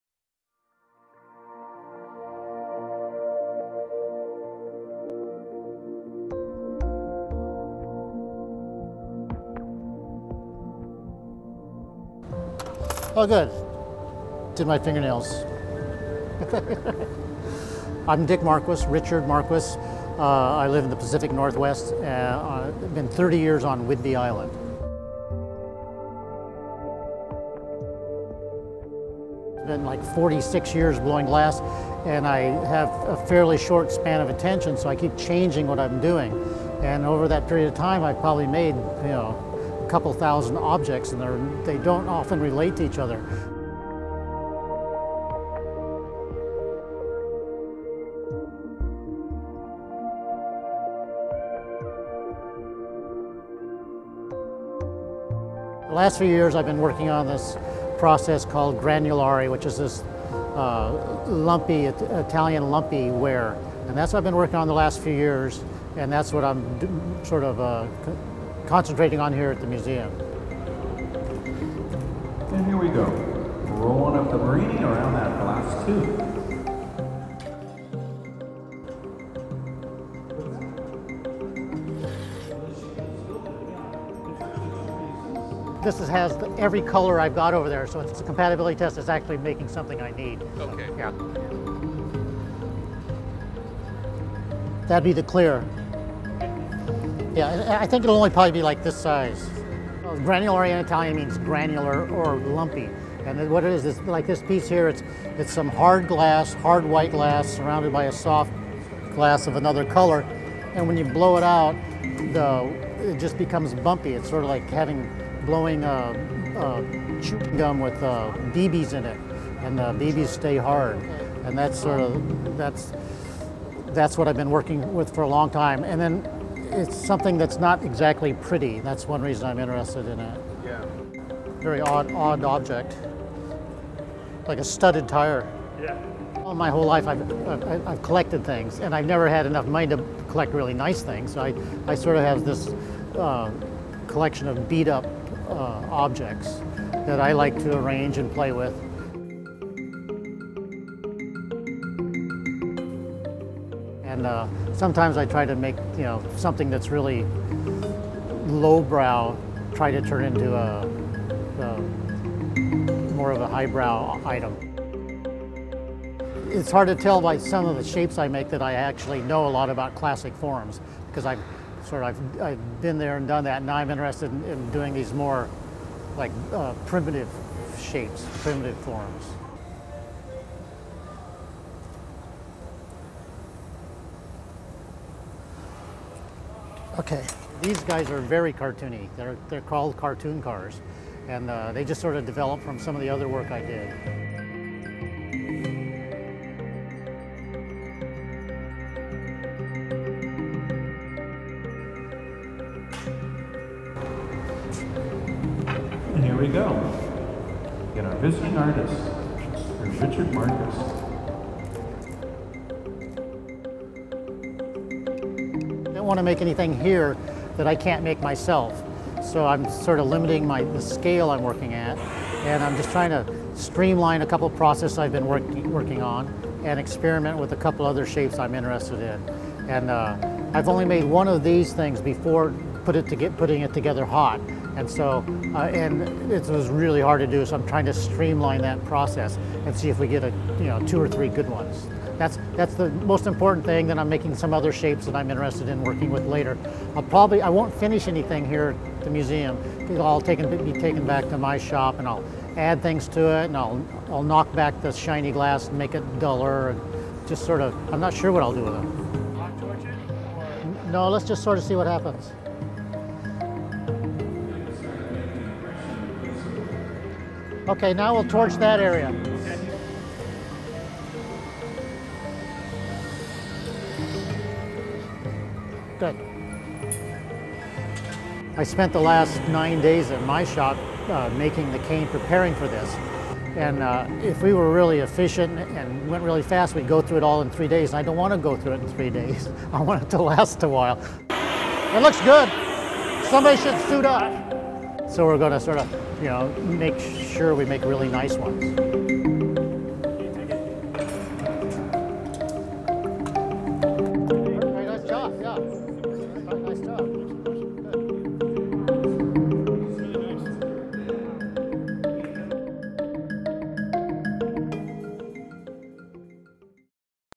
Meet the Artist: Richard Marquis
Visiting Artist Residency: Richard Marquis
Dates of the residency: December 5-9, 2012
All images courtesy of the artist. Directed by Derek Klein
Executive Director: Susan J. Warner
Museum of Glass Hot Shop Team: Benjamin Cobb, Gabe Feenan , Niko Dimitrijevic and Sarah Gilbert
Museum of Glass Hot Shop Interpretive Staff: Greg Owen, Walter Lieberman

Richard Marquis is a pivotal figure of the contemporary Studio Glass movement. Throughout his career he has freely shared his knowledge, demonstrating and teaching in the United States, Europe, Japan, Australia and New Zealand.  His work has a whimsy and spontaneity that belies the sophistication and depth of his artistry and has a distinctive sense of humor.